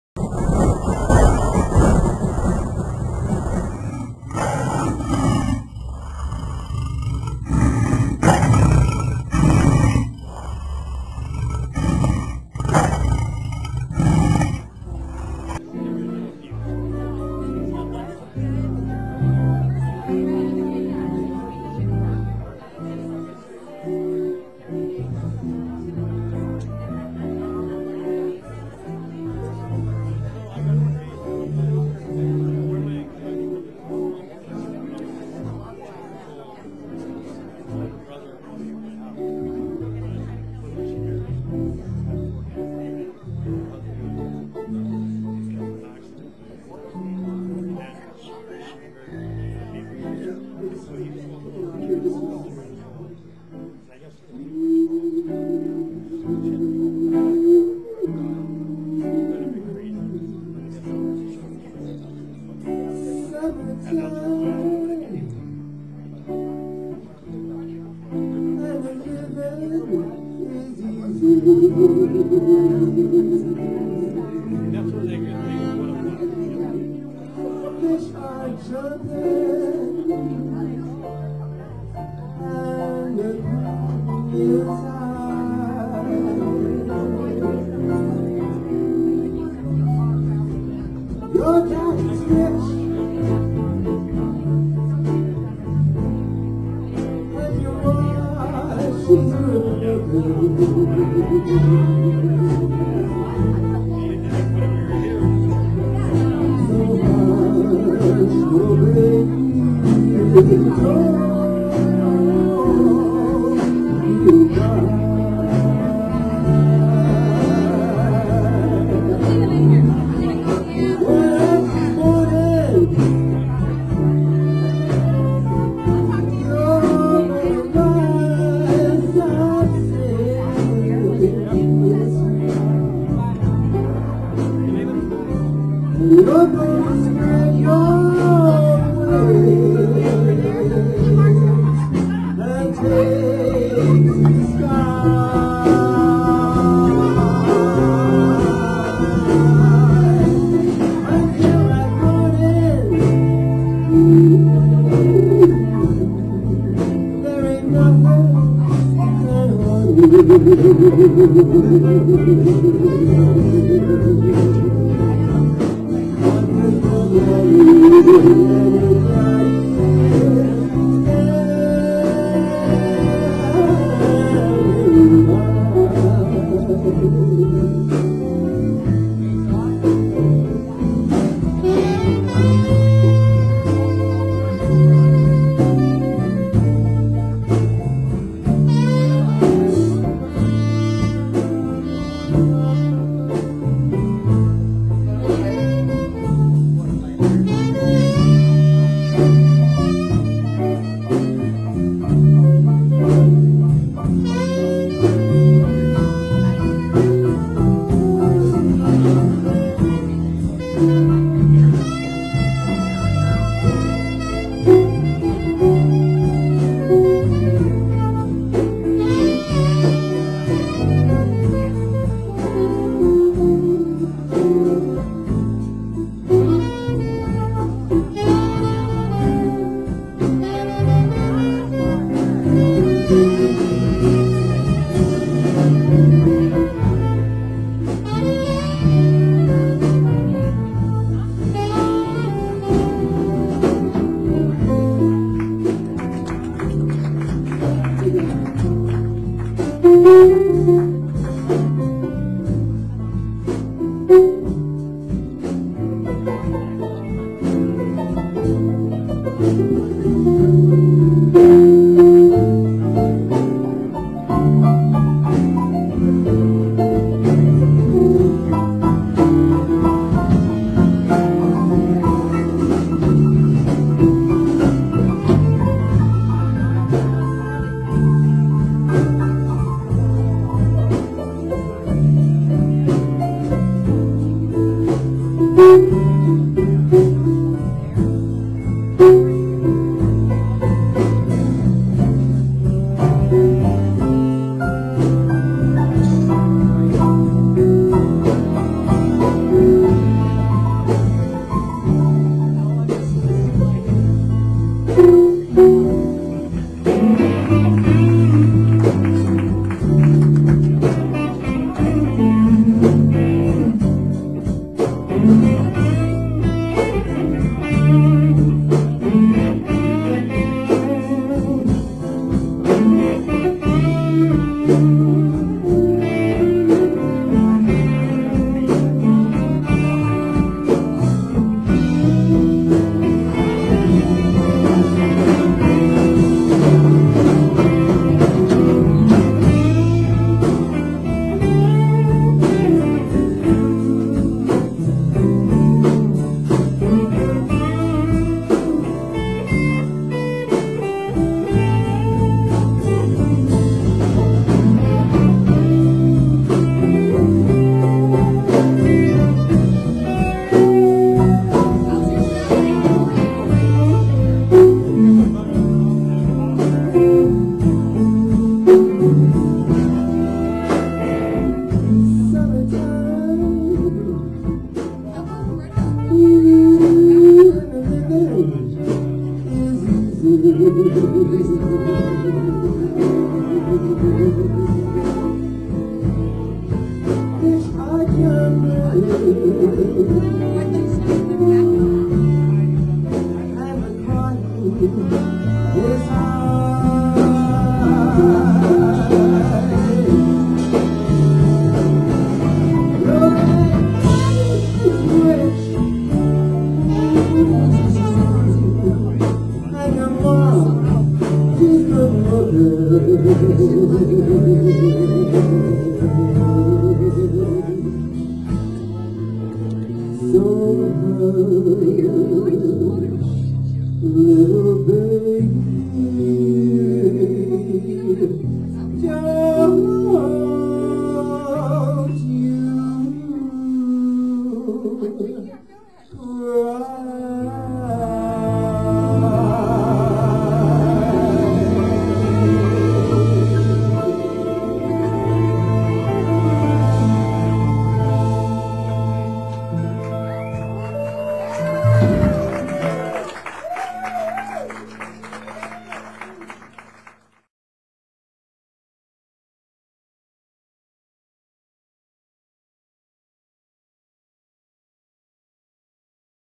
Lovely On Location: Blues Underground Jam
Haunting vocals, a smooth sax, tossed in with a delicious ensemble make this rendition of a classic unforgetable!